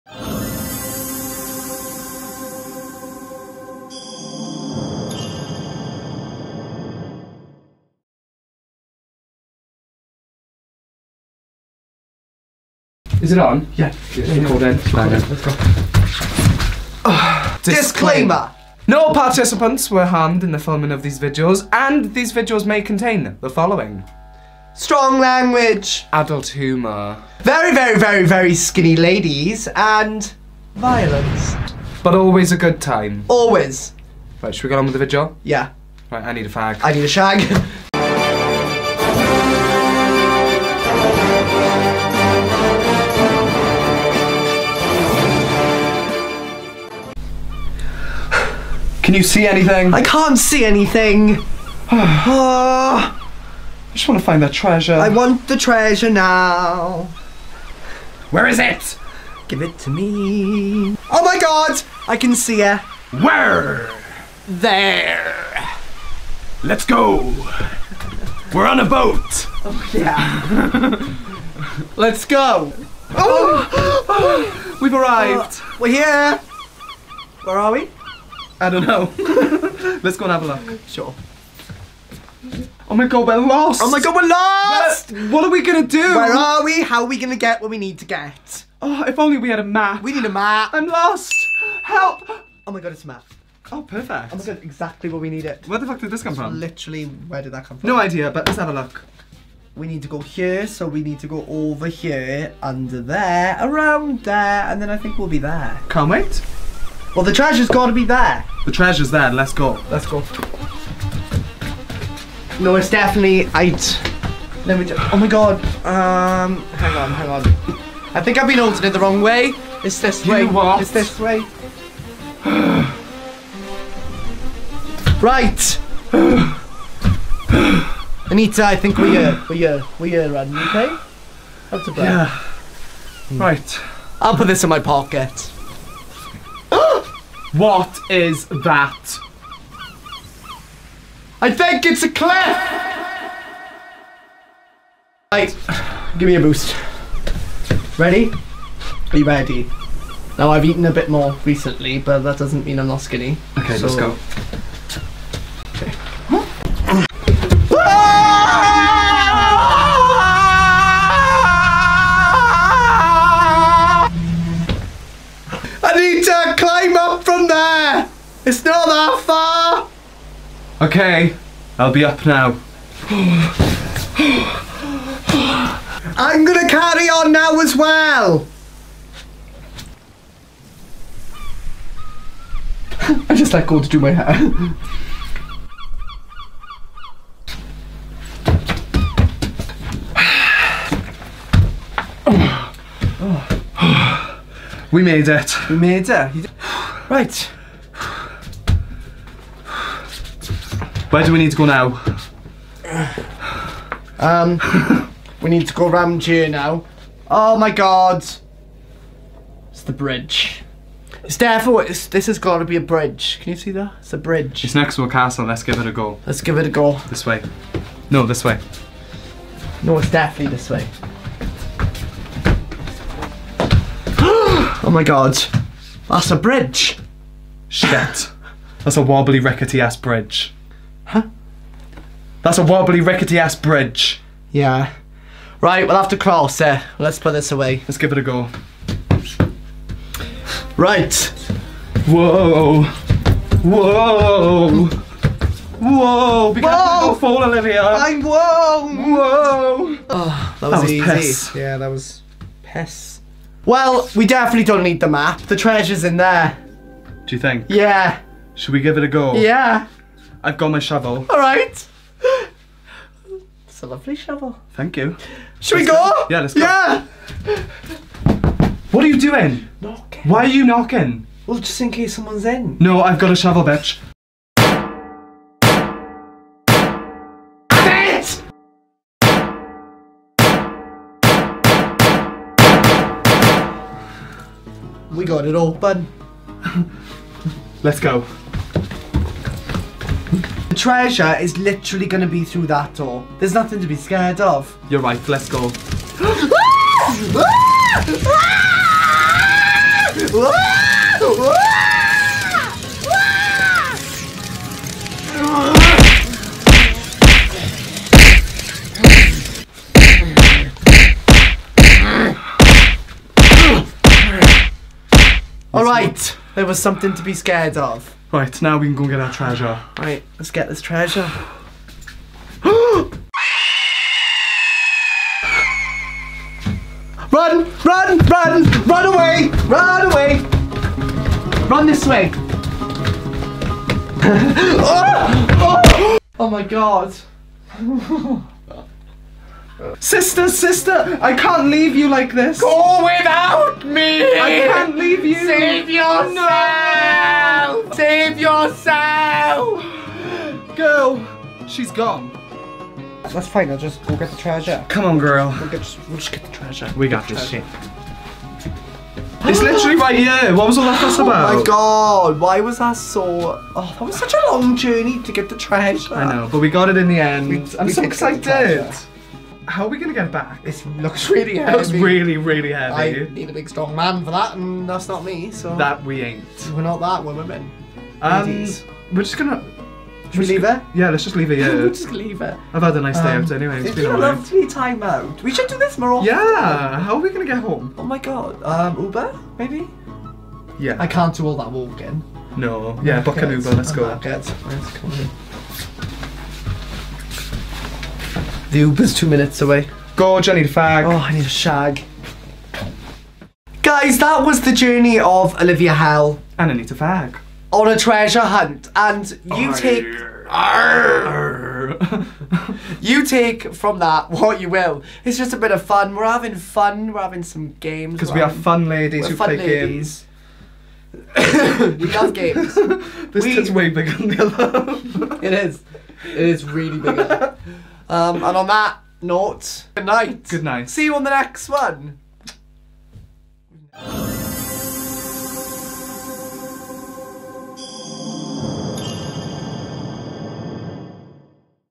emmaheaven.com: Life's A Drag October 2024
The magazine is excited to welcome superstar Drag Queens, Olivia Hell & Anita Fag.
Life's A Drag - so come join the party!
This is not a stand alone video - it is part of a feature in the emmaheaven.com online magazine.
This video contains strong language and adult content